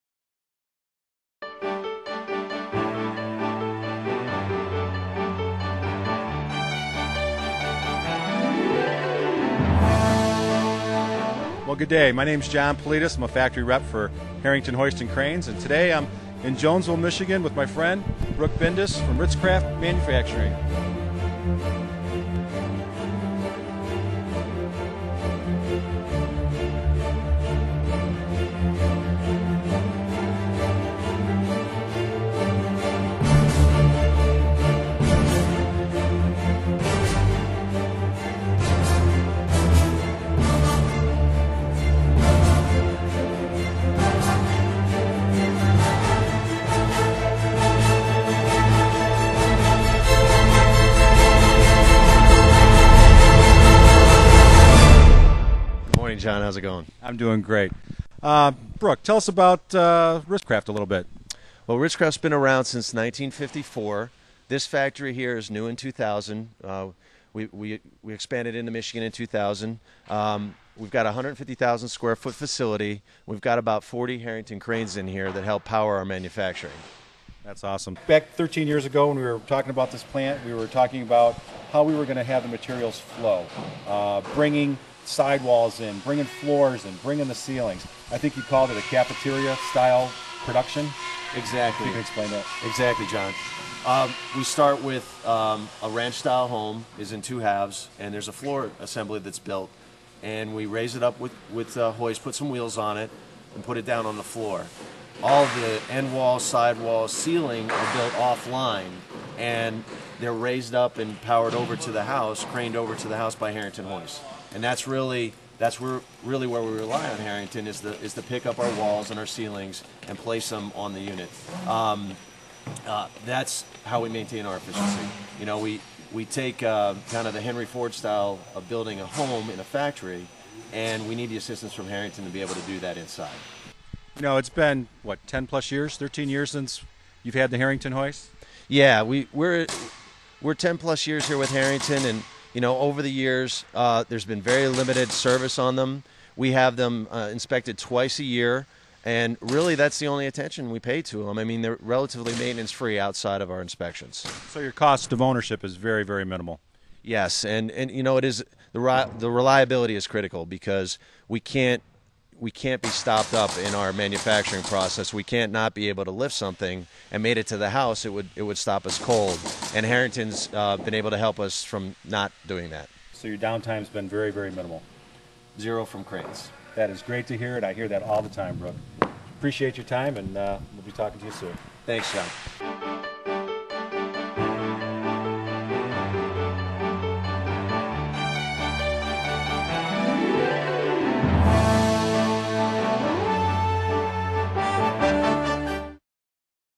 Industrial Hoist and Crane Systems: The Material Handling Choice for Green Ready Housing
Brook Bindus of Ritz-Craft Homes and John Politis of Harrington Hoists, Inc.